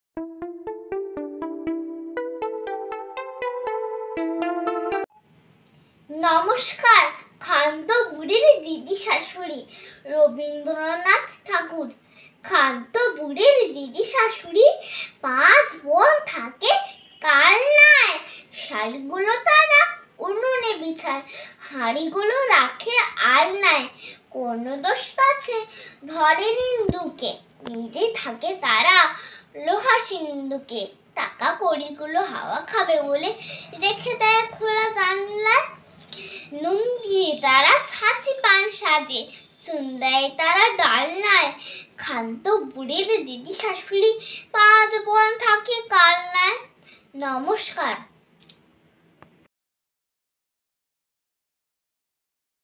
কবিতা। ক্ষান্তবুড়ির দিদিশাশুড়ি। কবি। রবীন্দ্রনাথ ঠাকুর। আবৃত্তি। অস্মিতা ধাড়া।
কবিতা। ক্ষান্তবুড়ির দিদিশাশুড়ি।

 আবৃত্তি। অস্মিতা ধাড়া।